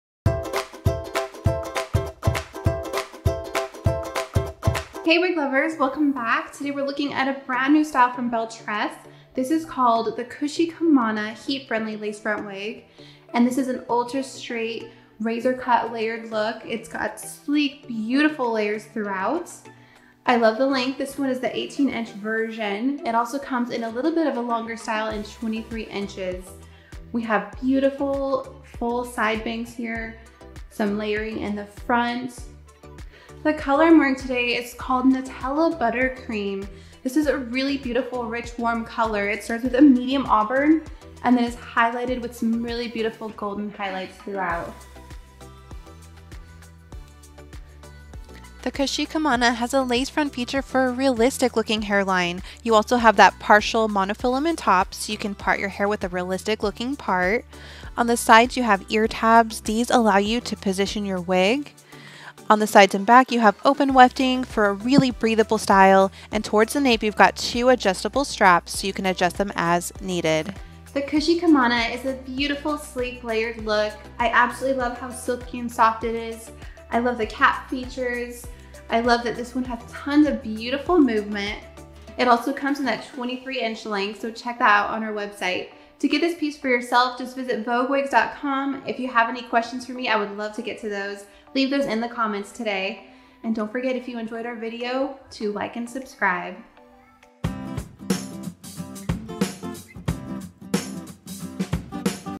Belle Tress Kushikamana 18 "| Voguewigs | Shay's Reviews
Hey Wig Lovers! This is the perfect mid length look with straight layers and heat-friendly fibers!  The hair is soft and silky and salon finished looking straight out of the box!  This lovely style is a great length for daily wear and you can change your look around with the heat-friendly option! ( Please don't forget to use a low to medium setting on your hot tools and your set!)